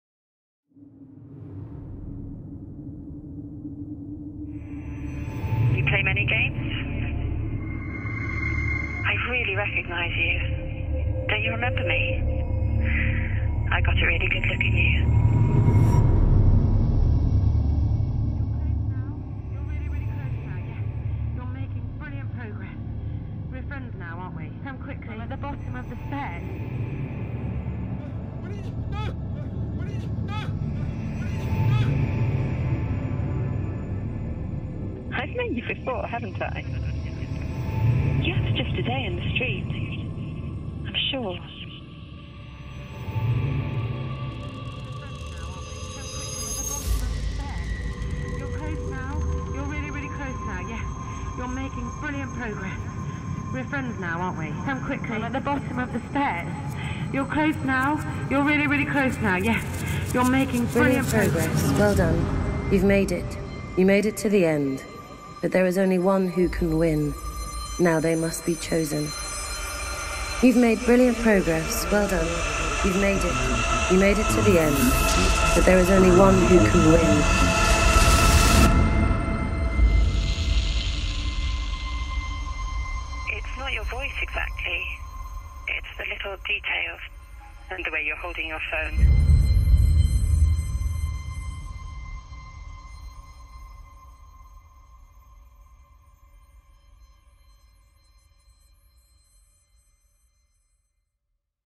Silverpoint
In April 2015 Absolut collaborated with Punchdrunk in an R&D Digital experiment where the lines between the digital and real world were blurred. Here’s a taste of what happened!